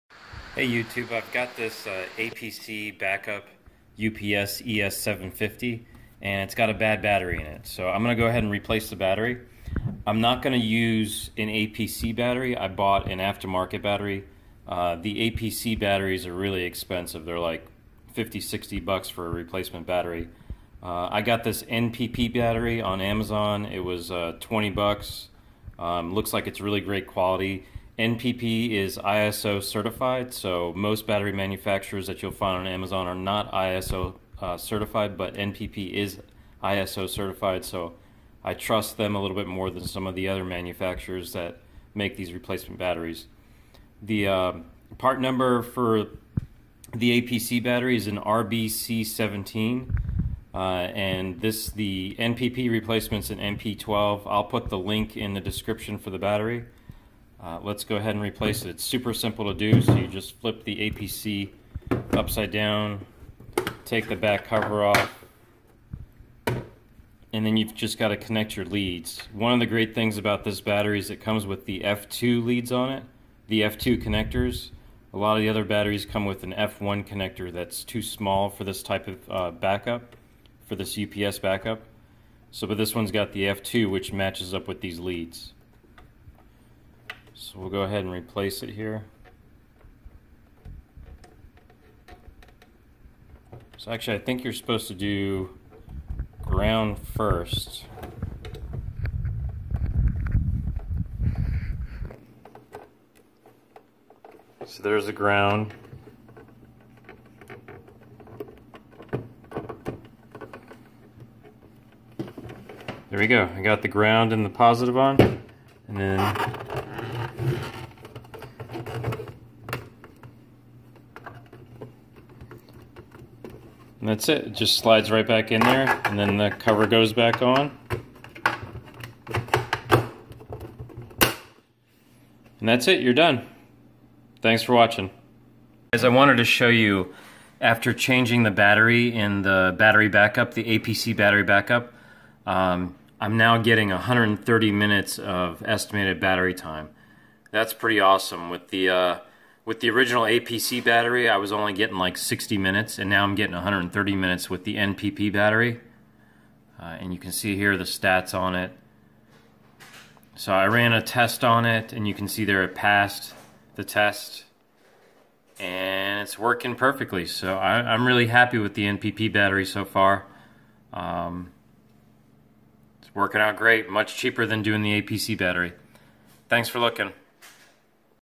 APC BE750G UPS RBC17 Battery Replacement and NPP Battery Review NP12-7Ah 12V 7Ah AGM F2 Terminals
Make sure you get the right terminal size. F2 terminals are 1/4" wide, F1 terminals are smaller at 3/16".